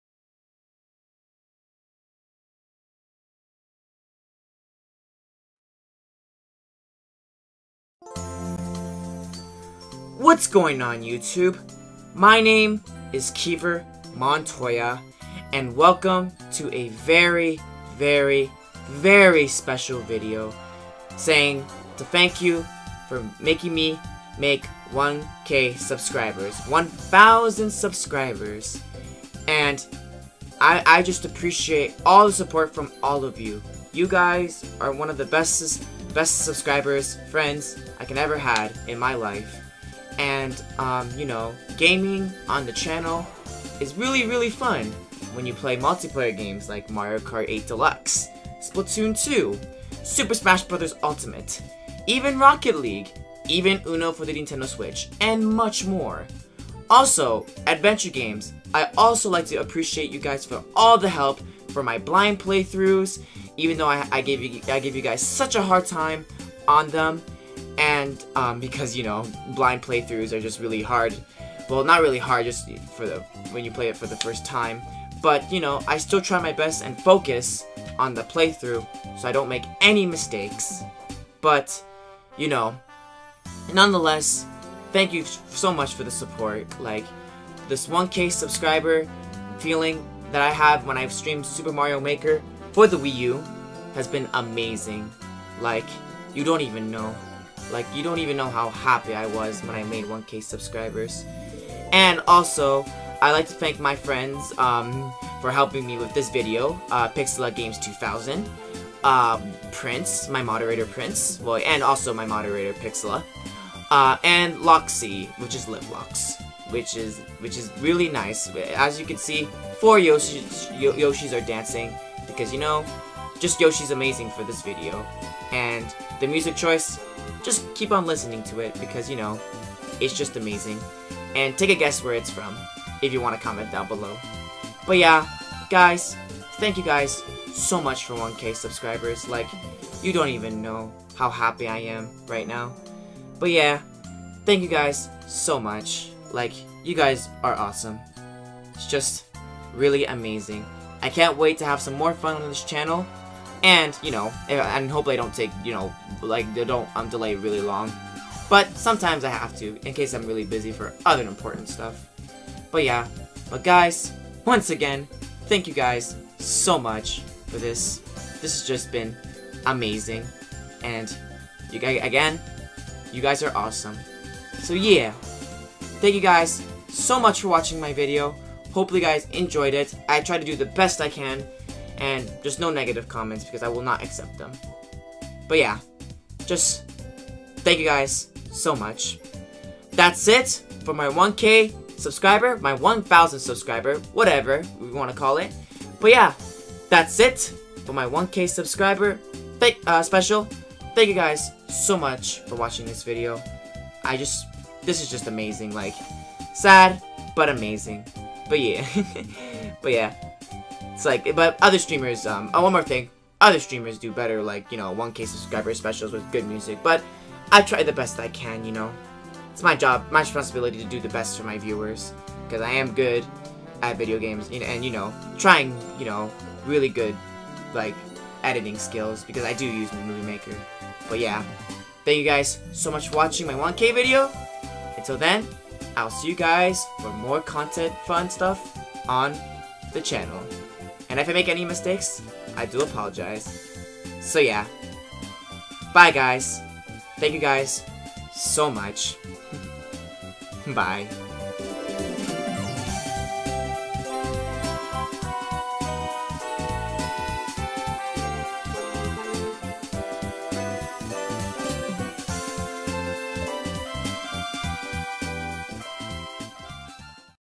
Kever's 1k Special Video :))
I did the best I can for this video and I like to say thank you guys for all the support you guys are truly amazing :))

PSA: Im Very Sorry This is late :)) But Idc

Player's to thank

Pixelagames2000
Prince
Liveloxs

YOSHI SQUAD XD

Edited on: Windows Live Movie Maker

Wii U: KeverMontoya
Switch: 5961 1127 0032
Skype: Kever Montoya